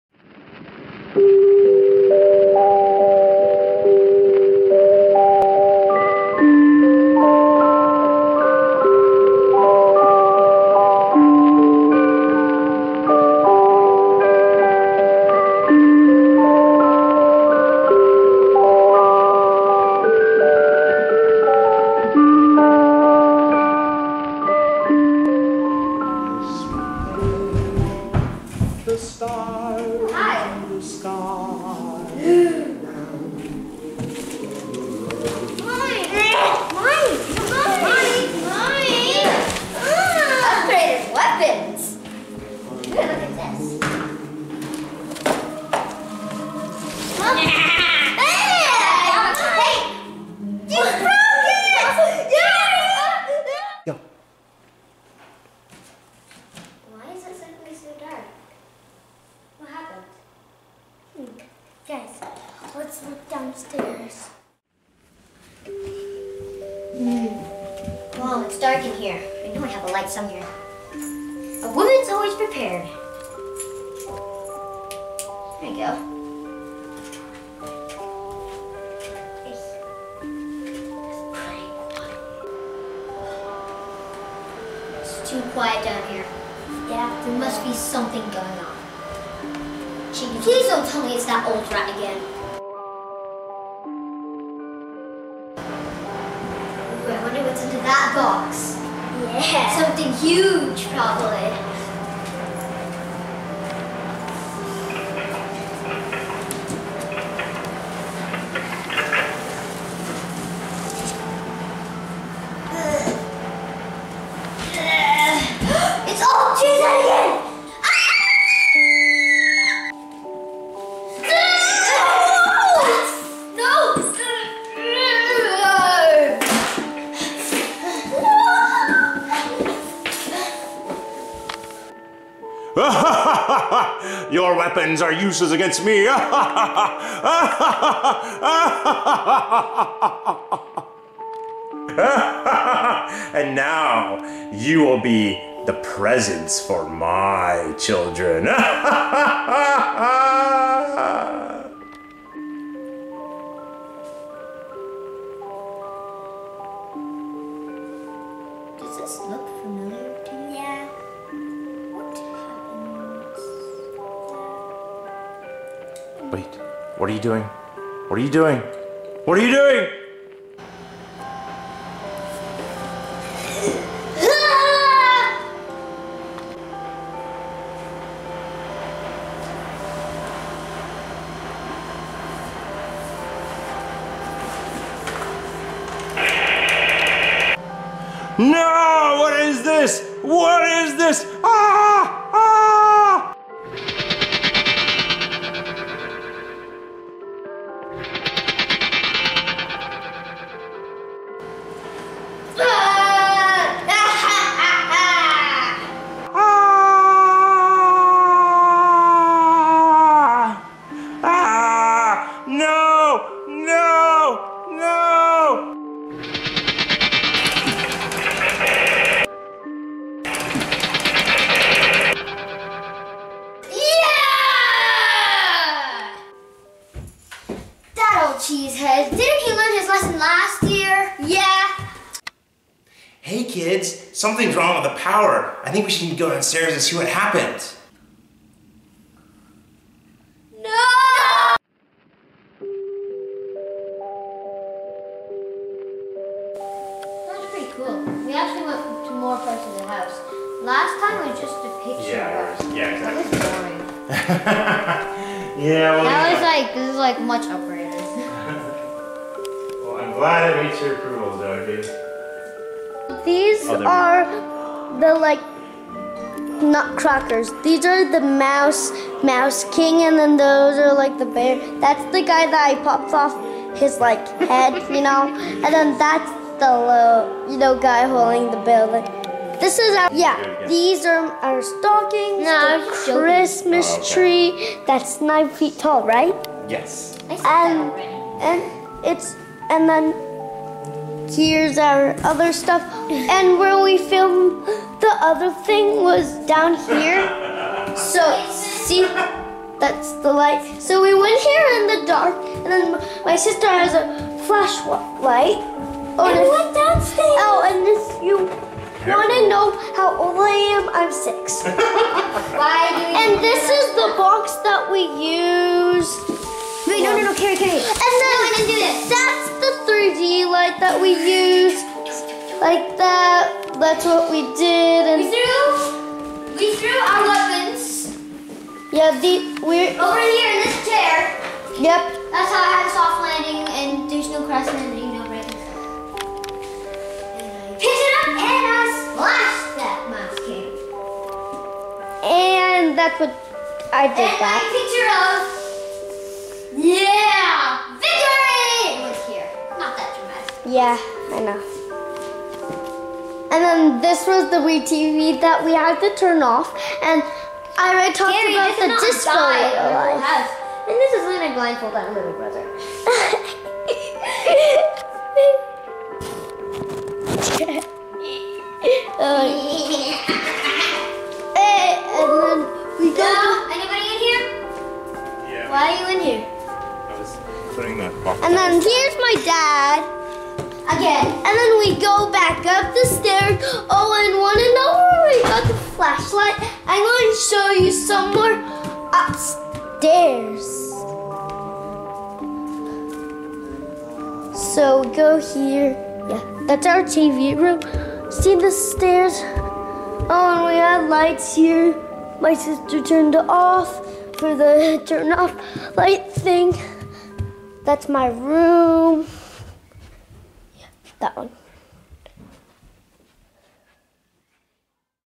Nutcrackalypse II: The Crackening
If you liked the original Nutcrackalypse, you'll love this one!

Follow Jackie and Cary as they battle the evil Mouse King.